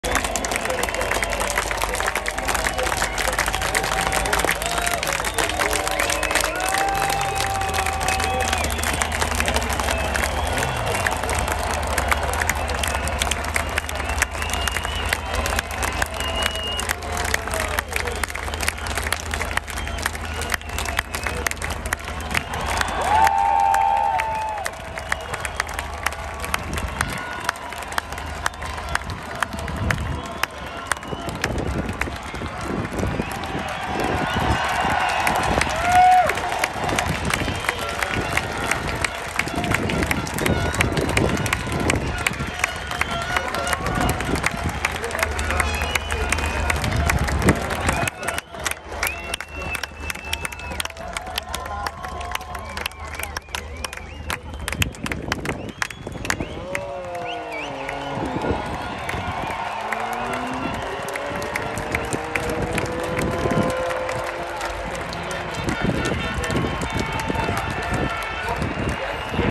MotoGP Jake Dixon applause Silverstone
Great reception for Jake Dixon after his fine P3 in Moto2 at Silverstone